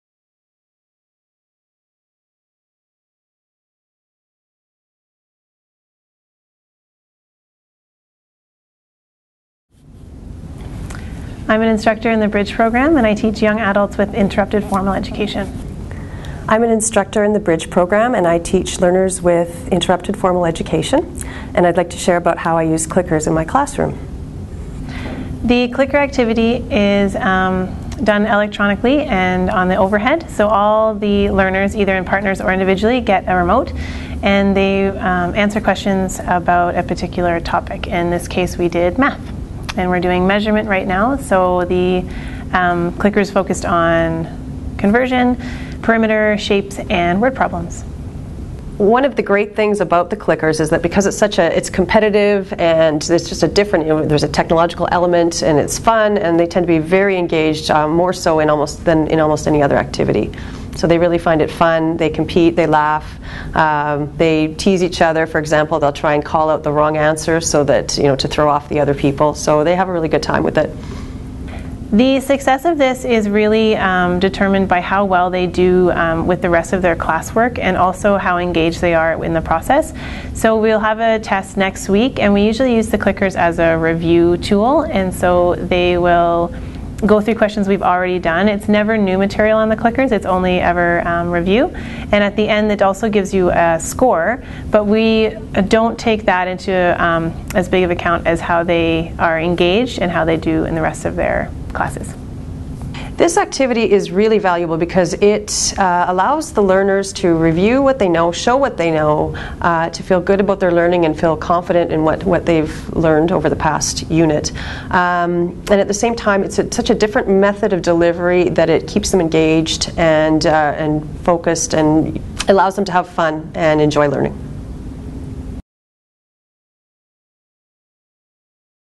Using Clickers in the ESL Literacy Classroom
This activity took place in a beginning-level math class for immigrant youth with literacy needs. The activity was a jeopardy game using CPS technology (clickers).